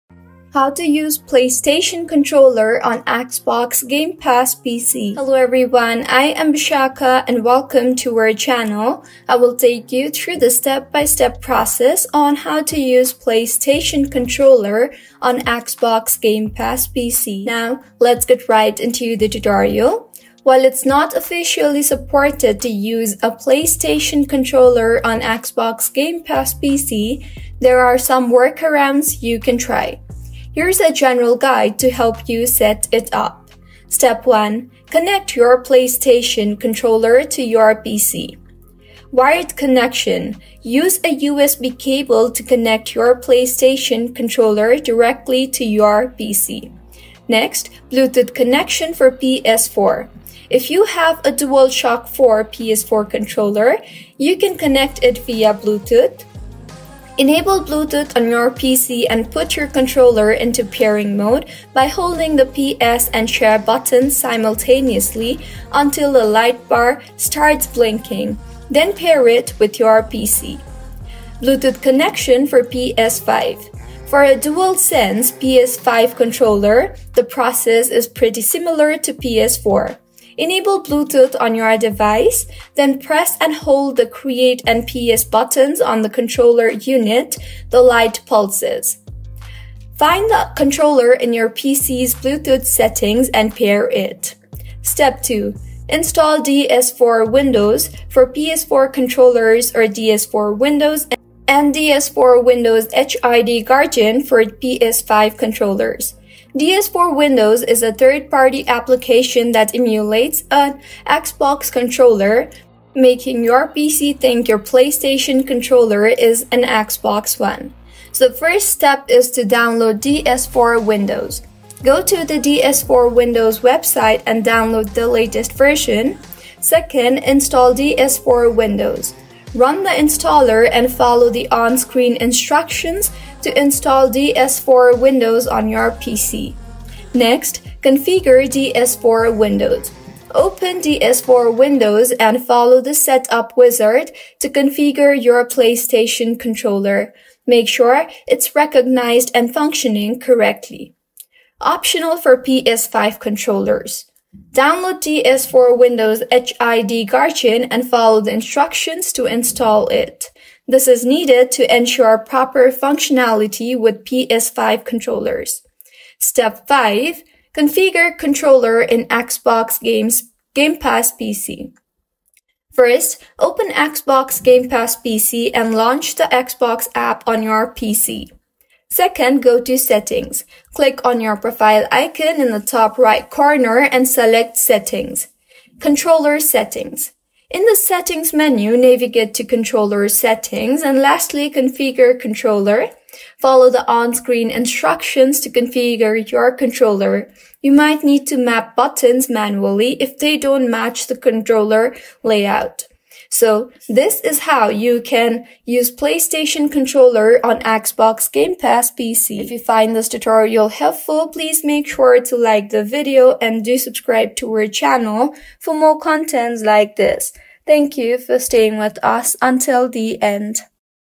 How To Use Playstation Controller On Xbox Game Pass PC (PS4 & PS5) - Full Guide
Learn how to use a PlayStation controller on Xbox Game Pass for PC with this comprehensive guide. Follow these step-by-step instructions to connect your PS4 or PS5 controller and enjoy your favorite games.

In this tutorial:
0:00 - Introduction
0:05 - How To Use Playstation Controller On Xbox Game Pass PC (PS4 & PS5) - Full Guide

Discover how to connect and use your PlayStation controller on Xbox Game Pass for PC. This tutorial covers the entire process, ensuring you can seamlessly integrate your PS4 or PS5 controller for a better gaming experience.

Enhance your Xbox Game Pass PC experience by using a PlayStation controller with this full guide!